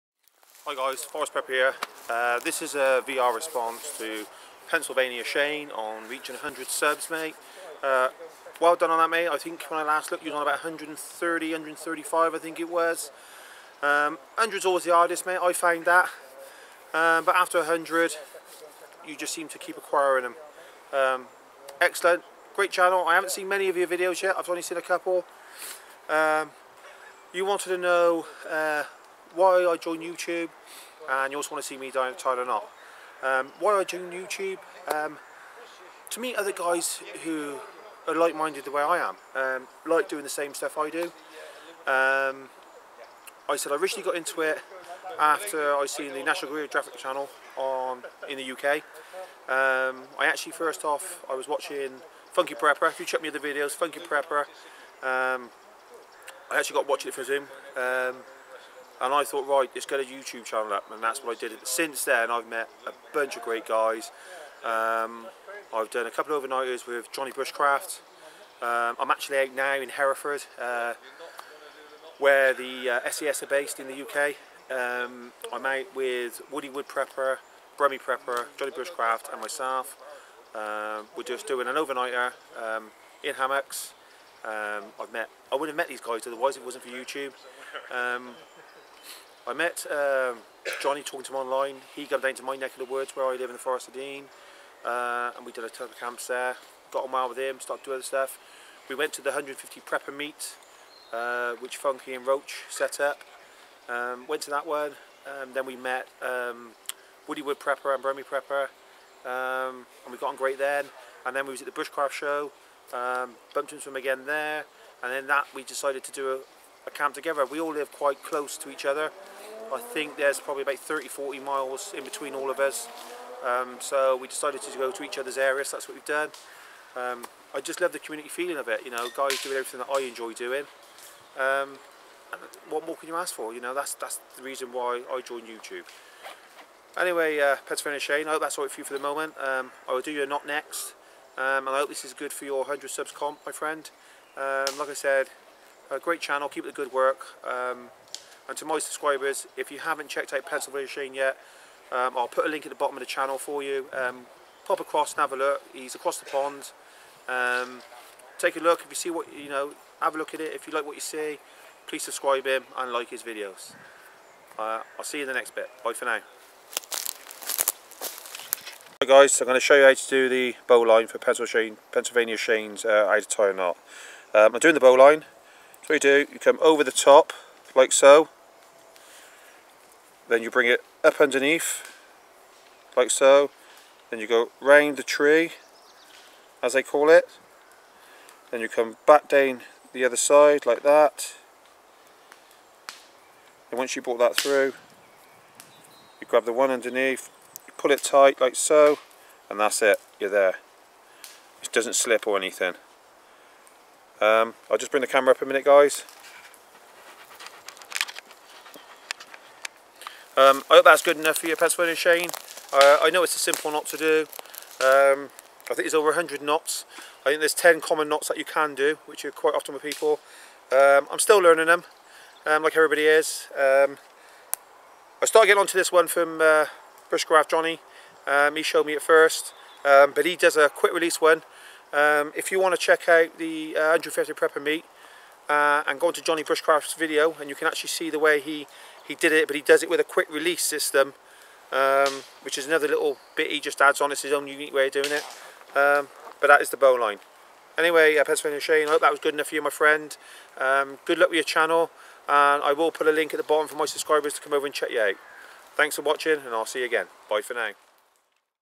Pennsylvania Shane 100 Subs VR
VR on Why i got into utube & why i love it & Showing your Favorite Knot.